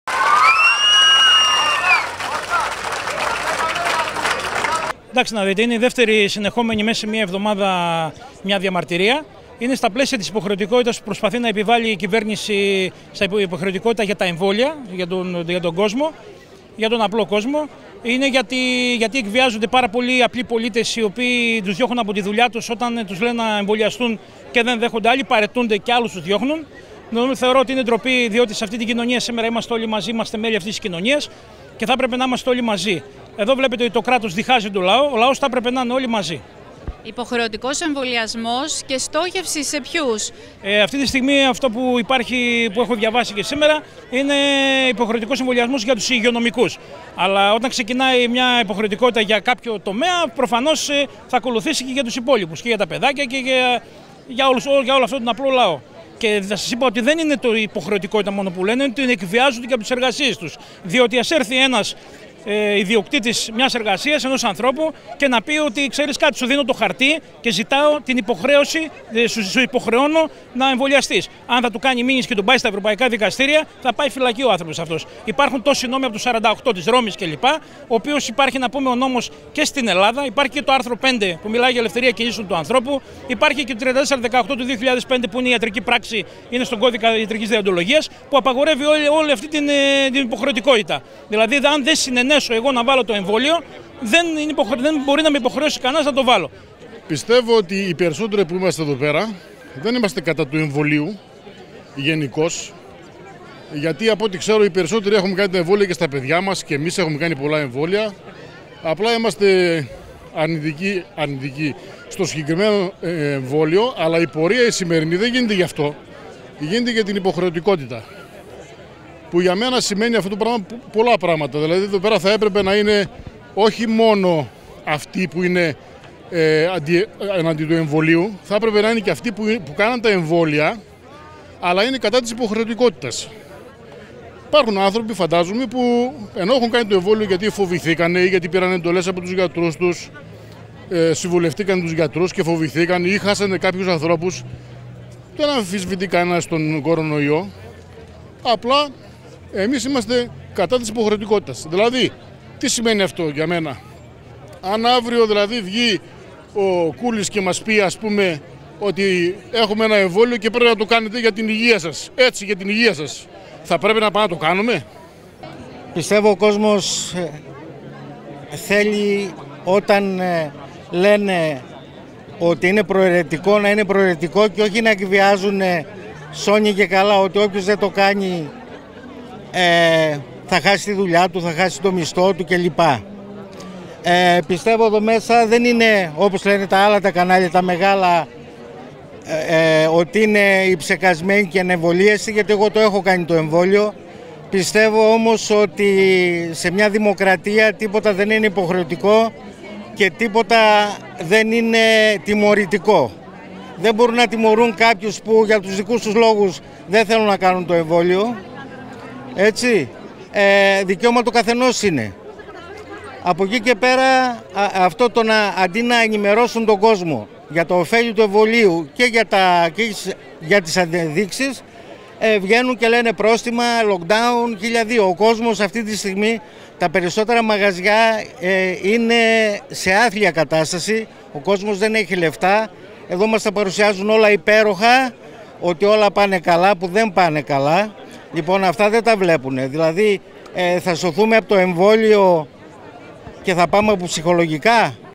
Συγκέντρωση και πορεία κατά του Υποχρεωτικού Εμβολιασμού στην Πλατεία Ελευθερίας
Συγκέντρωση και πορεία κατά του εμβολιασμού πραγματοποιούν αυτή την ώρα αντιεμβολιαστές αλλά και εμβολιασμένοι οι οποίοι διαφωνούν με τον υποχρεωτικό εμβολιασμό.